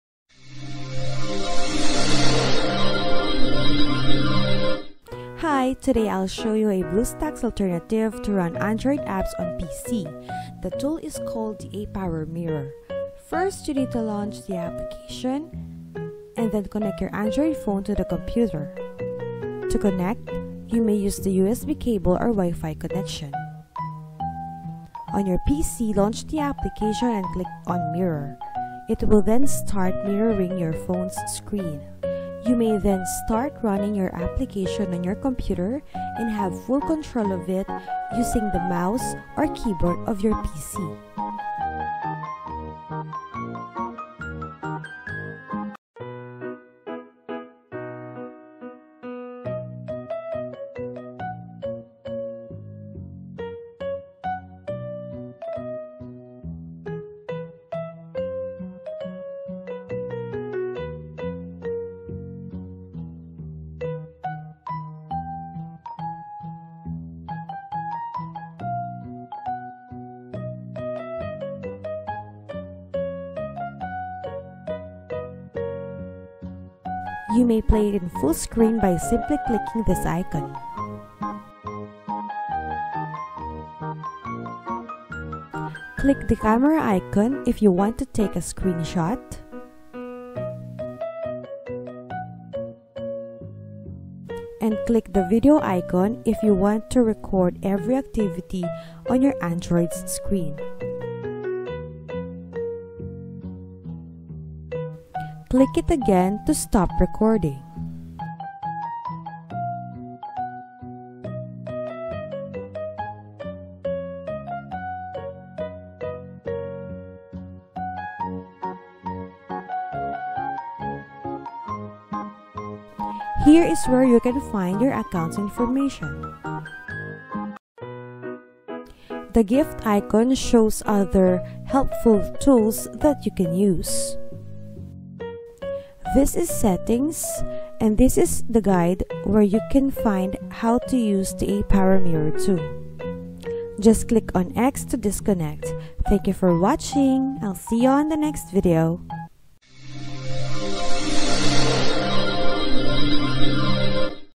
Sent BlueStacks Alternatives Best Tools to Run Android Apps on PC
Want to run Android apps on PC without using BlueStacks? In this video, you will know the best tool to run Android apps on PC.